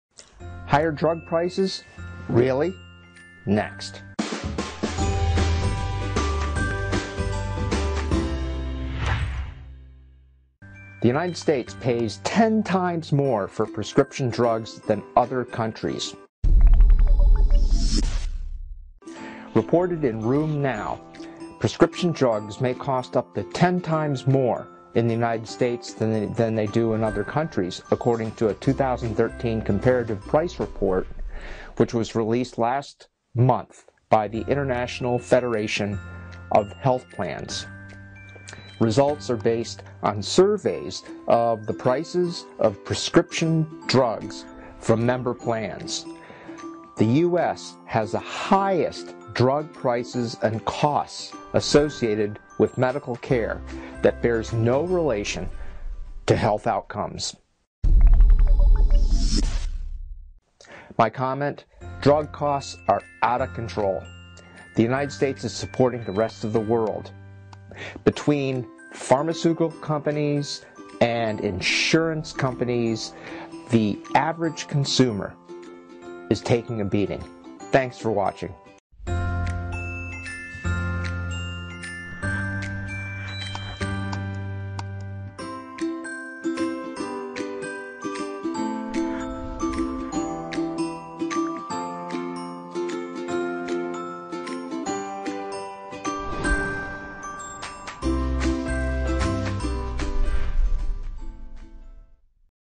High Drug Prices
High drug prices… really?  Next

U.S. Pays 10 Times More for Prescription Drugs than Other Countries

Reported in Rheum Now, prescription drugs may cost up to 10 times more in the United States than they do in other countries, according to a 2013 Comparative Price Report was released last month by the International Federation of Health Plans (IFHP). Results are based on surveys of the prices of prescription drugs from member plans.
The US has the highest drug prices and costs associated with medical care that bears no relation to health outcomes.

Comment: Drug costs are out of control.  The U.S. is supporting the rest of the world. Between pharmaceutical companies and insurance companies, the average consumer is taking a beating.